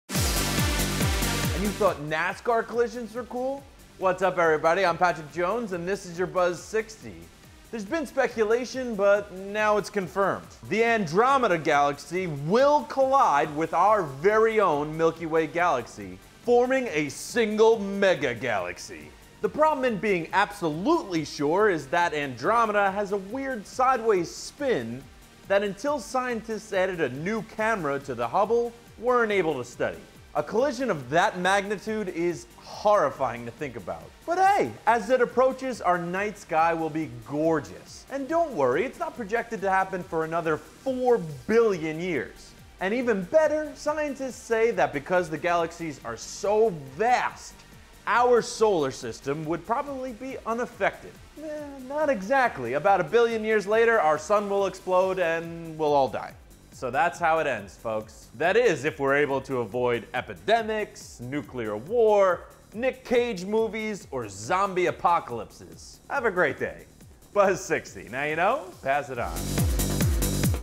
Galaxies Set to Collide, but Can Earth Survive?
Scientists are confirming what was once just a great sci-fi movie plot. The Andromeda Galaxy will collide with our Milky Way Galaxy, forming a single mega-galaxy. Don't cancel your weekend plans, it's not supposed to happen for another four billion years. But is it the end of Earth? Patrick Jones has the answer?